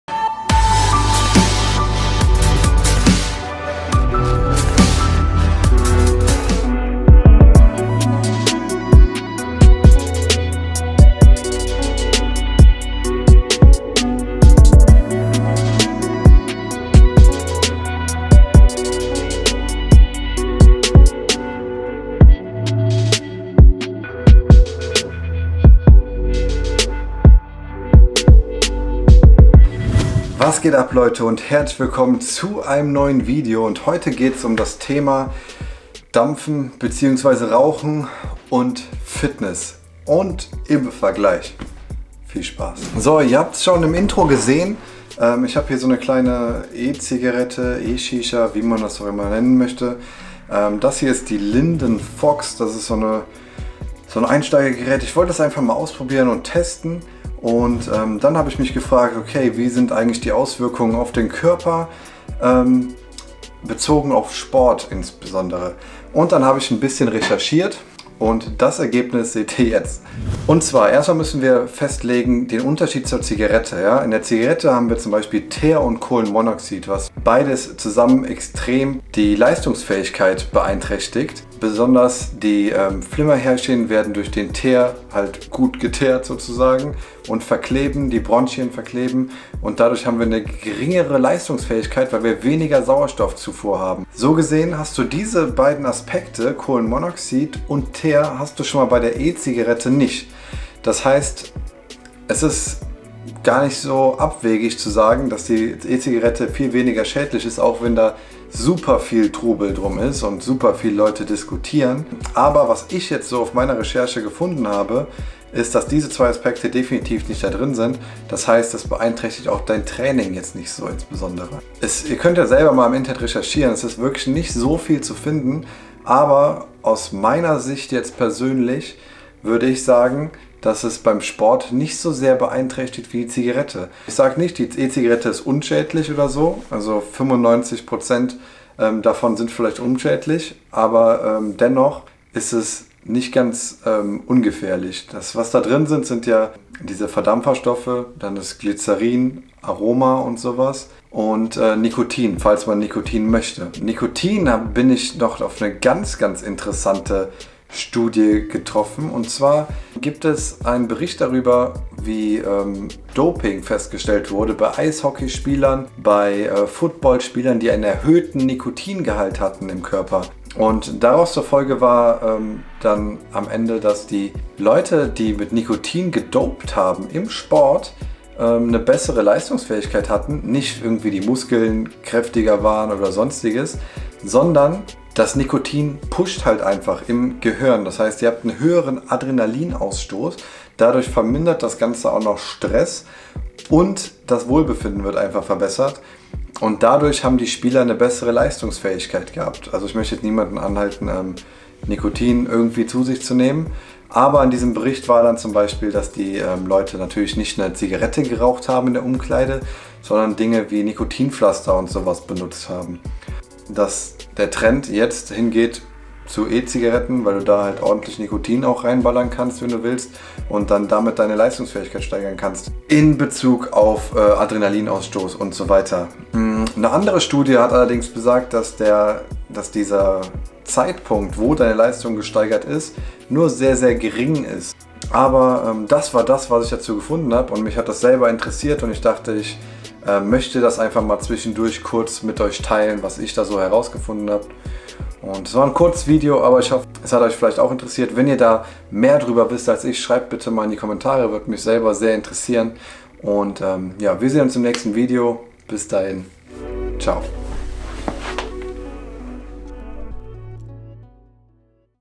Dampfen & Sport / Fitness Nikotin zur Leistungssteigerung
Ich habe mir eine E Zigarette gekauft und dann sind mir einige Fragen gekommen über die Auswirkungen auf den Körper besonders für Sportler für mich.
Was ich heraus gefunden habe erfahrt ihr in diesem Video.